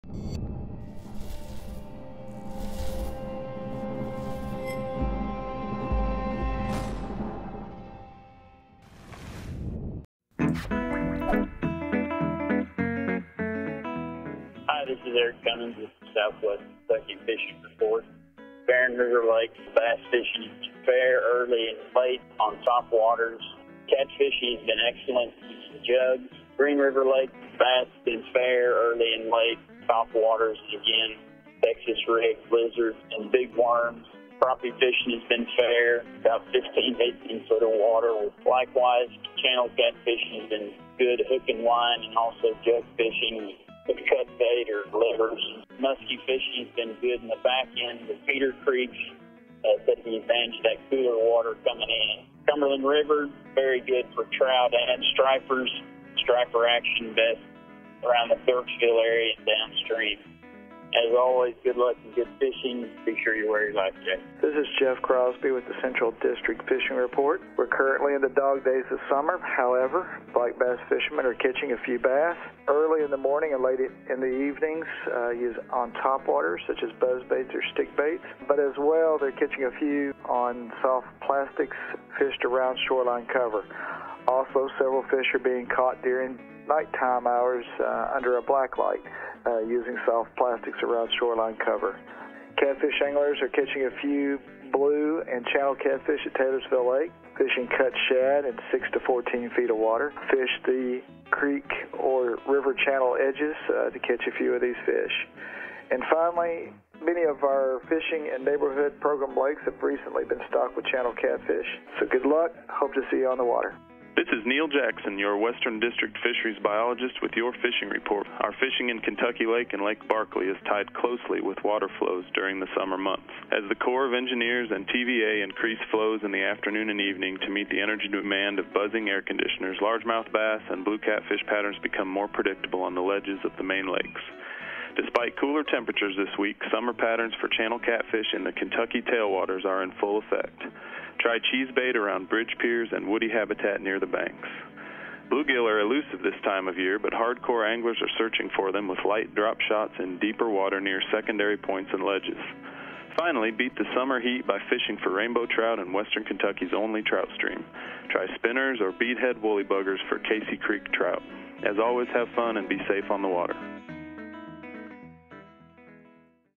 Fishing Report for August 2, 2014
Find out who's catching what and where this week in Kentucky waters